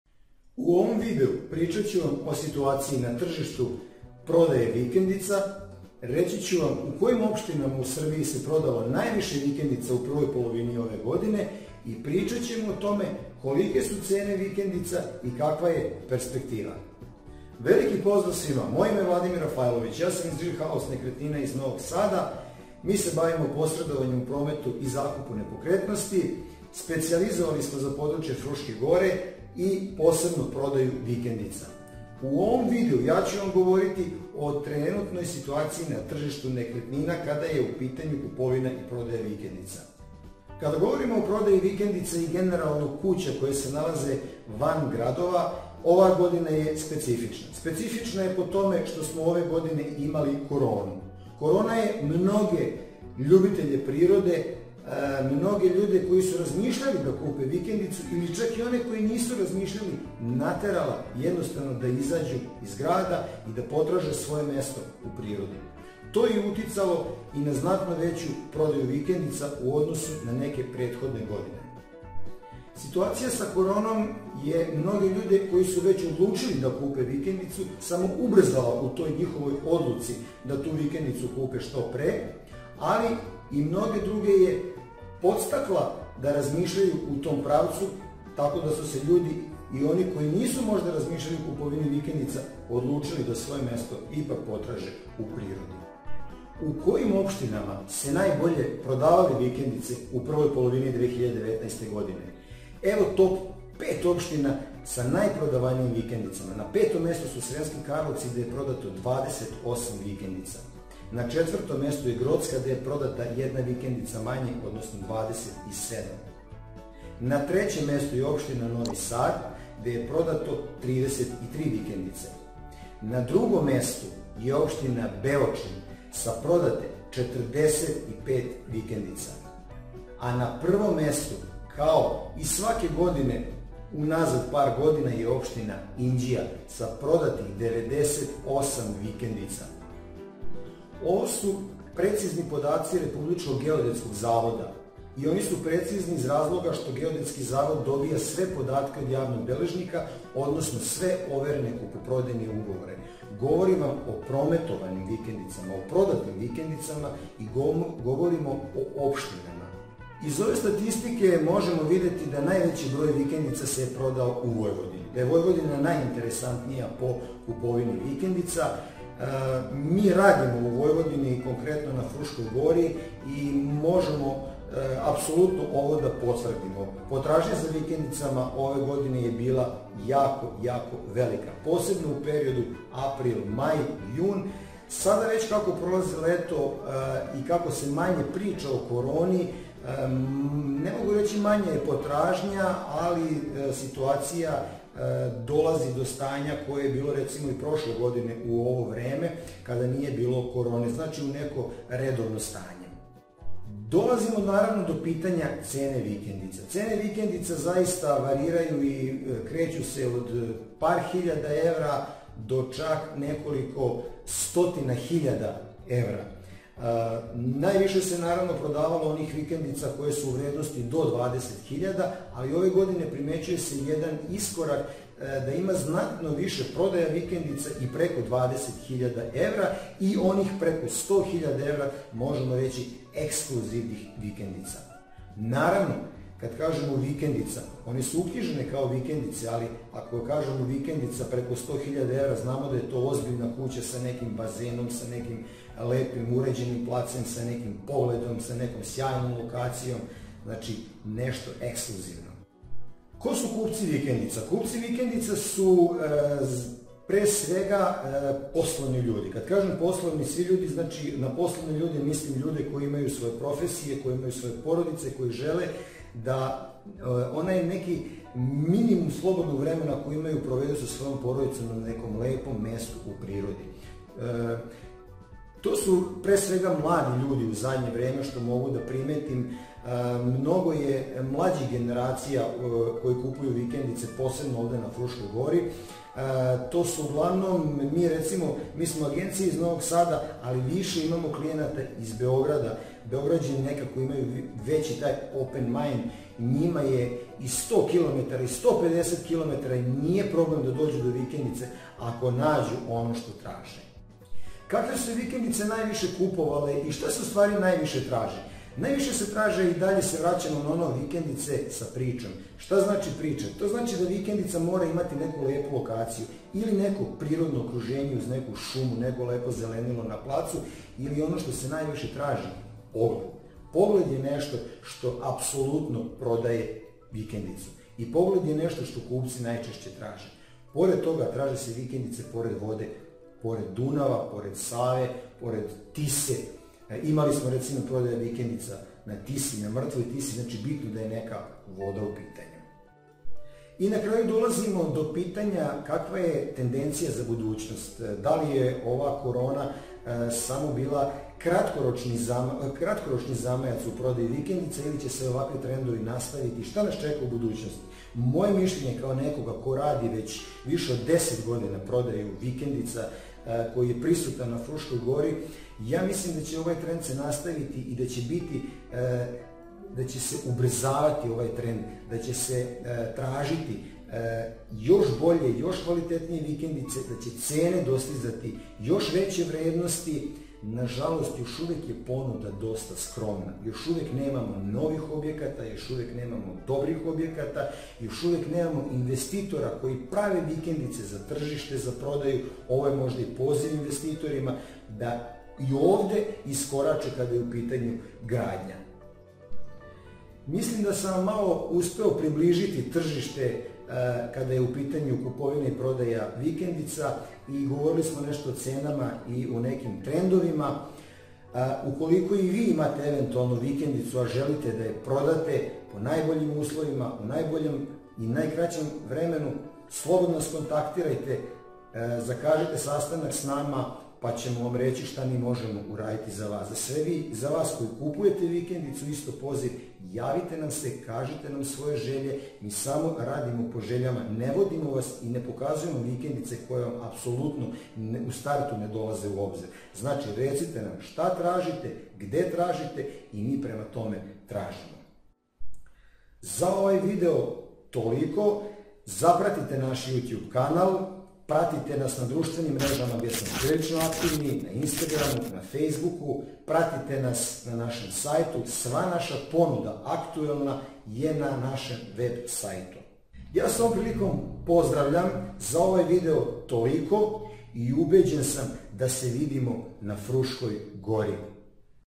Prodaja Vikendica - Koliko koštaju i gde su se najviše prodavale vikendice - Real House
Prodaja vikendica 🏠

Gde su se najviše prodavale vikendice ?
Zašto su vikendice toliko tražene i
Kako će izgledati tržište vikendica samo su neka od pitanju na koje odgovor možete dobiti u ovom videu.

Vikendice za prodaju i vikendice na Fruskoj gori iz naše ponude možete pogledati na našem sajtu: